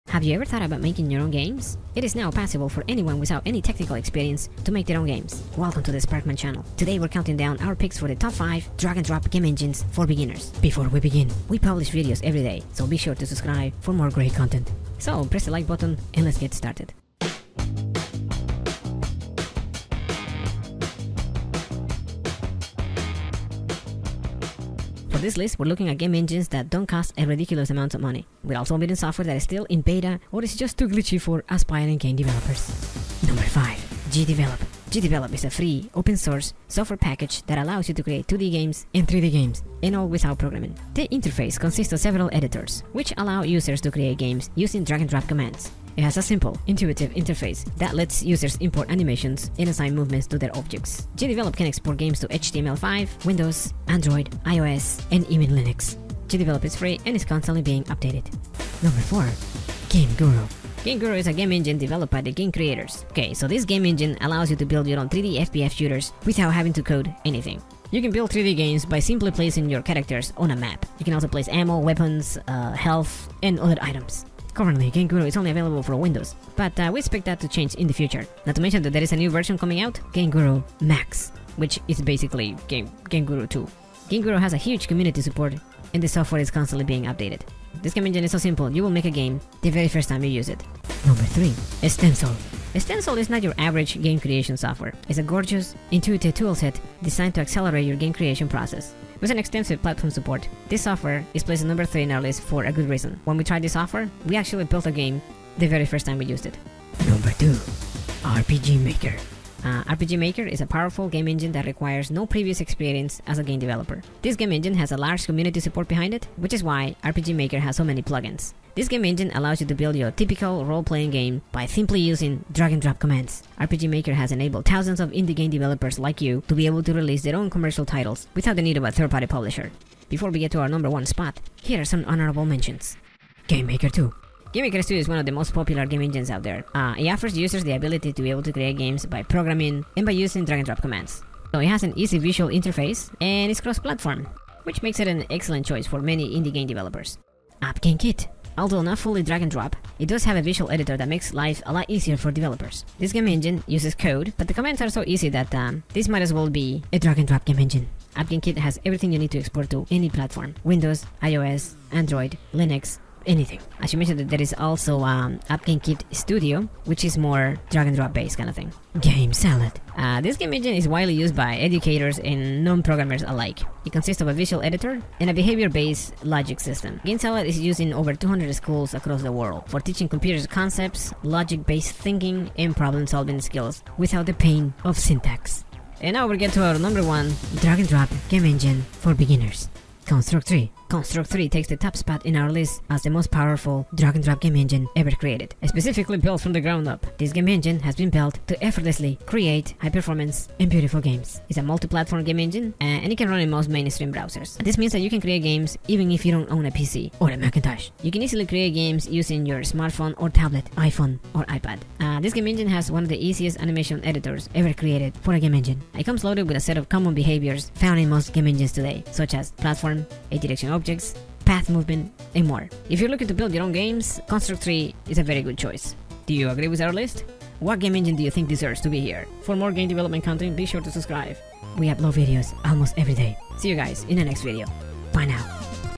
TOP 5 GAME ENGINES FOR BEGINNERS
Are you looking to get into game development? no programming skills? no problem, here are 5 game engines you can use to make your first game.

These game engines are the very best out there!

Do you like our content?

Gamemaker Studio 2